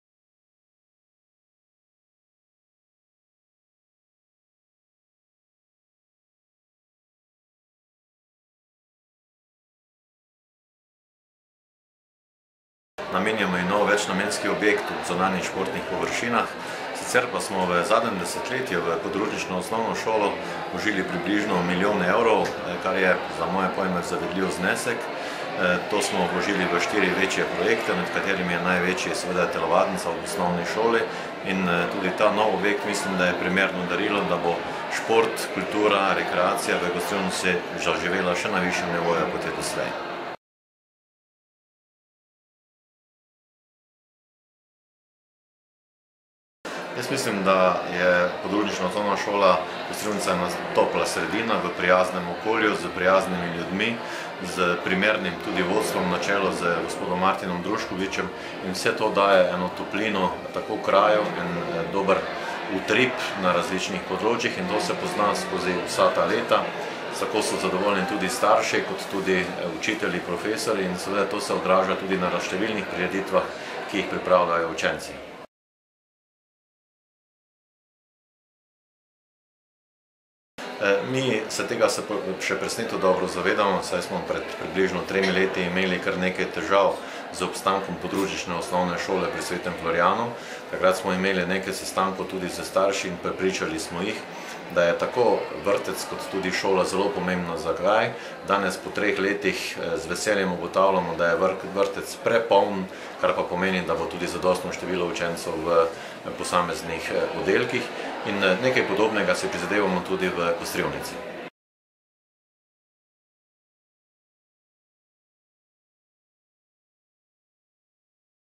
Župan občine Rogaška Slatina mag. Branko Kidrič o 200-letnici OŠ Kostrivnica
Župan občine Rogaška Slatina mag. Branko Kidrič je na proslavo ob 200-letnici OŠ Kostrivnica podal izjavo o darilu šoli in učencem ter učiteljem ter o ostalih zadevah.